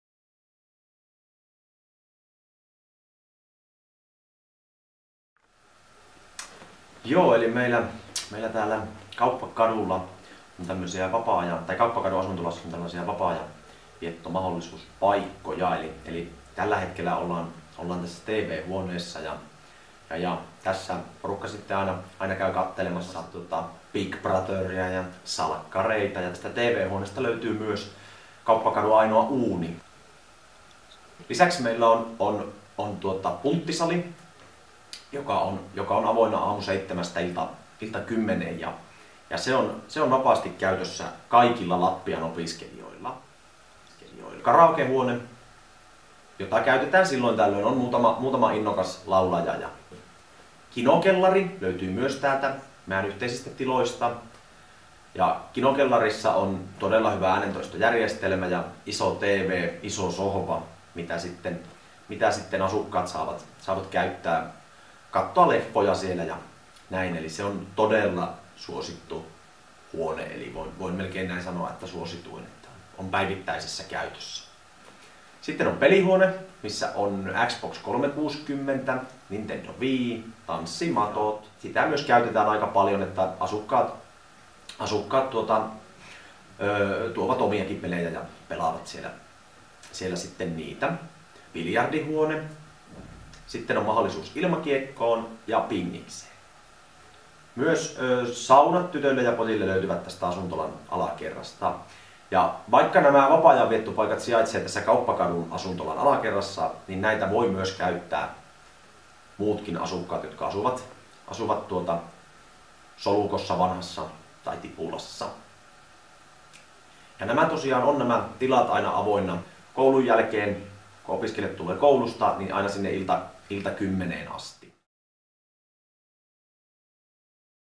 Tornion opiskelija-asuntolan vapaa-ajan tilat
Kauppakadun asuntolan alakerrasta löytyy monenlaista tekemistä opiskelijoiden vapaa-ajalle.